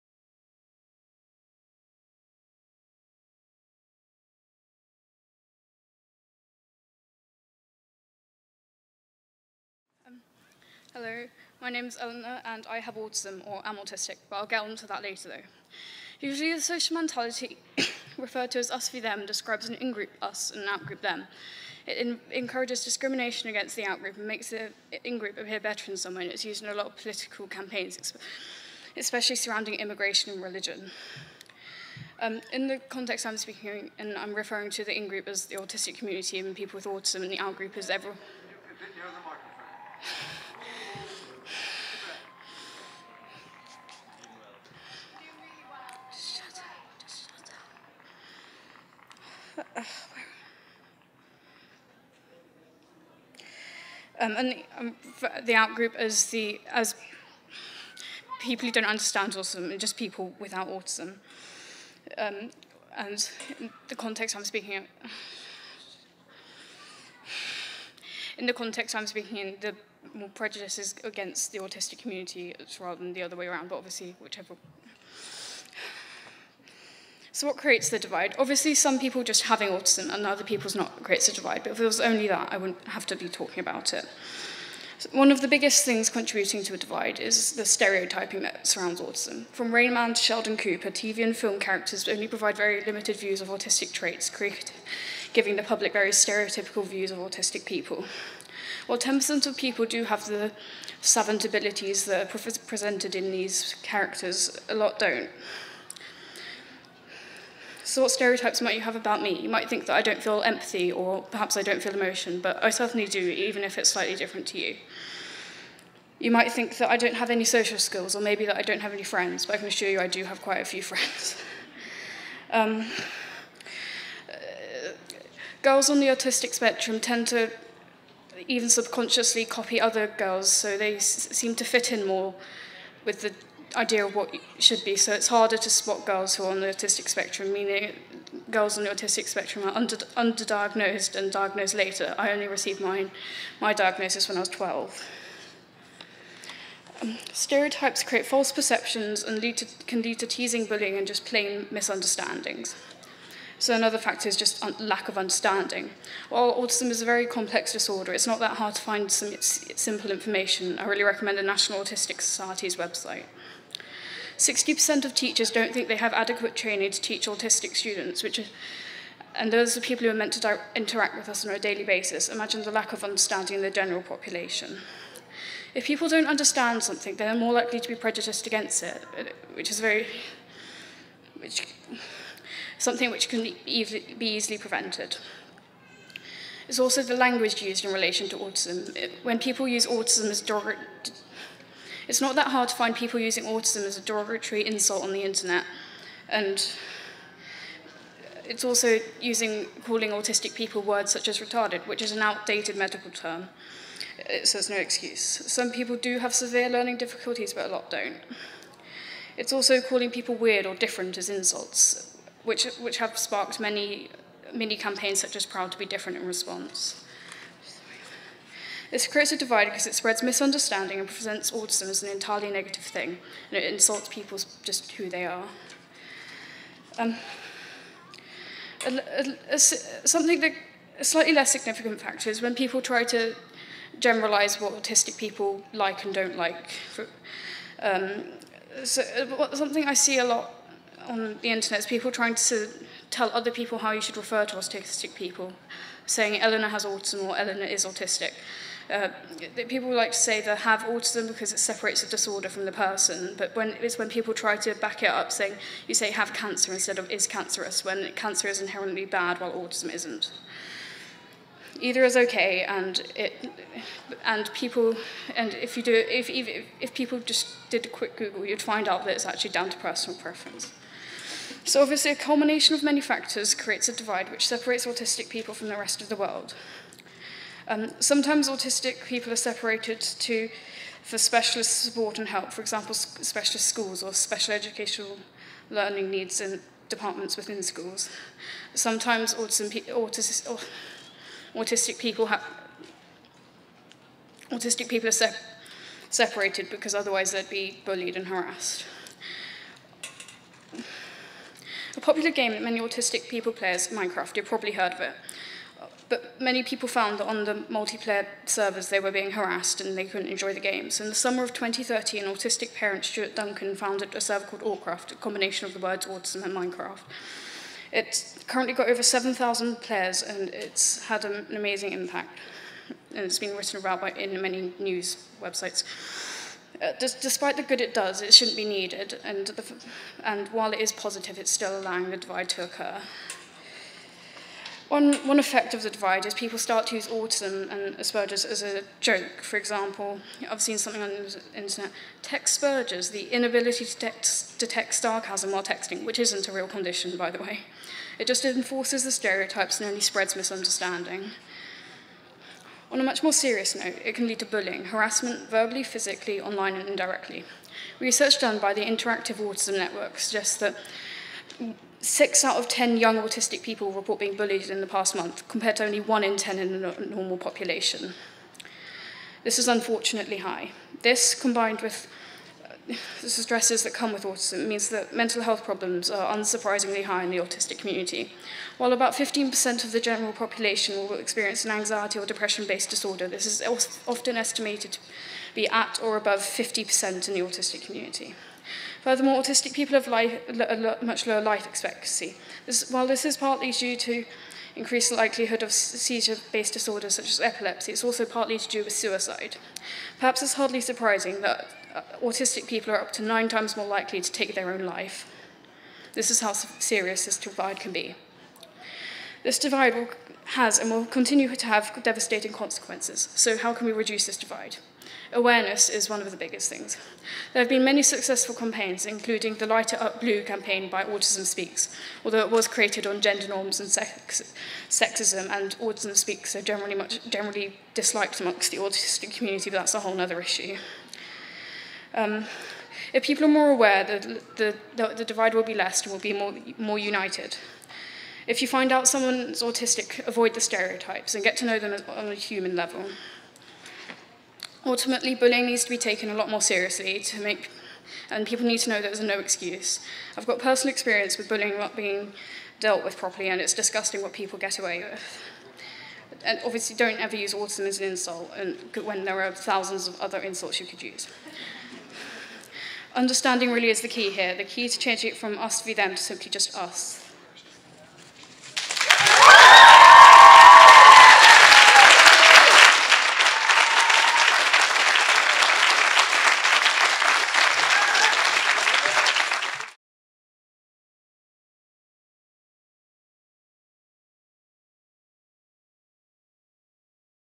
Autism: 'Us And Them' – Overcoming Prejudice
A talk given as part of our 'GTV Youth: Tell It Like It Is' filmed shorts at Greenbelt 2016 by Eleanor Coward.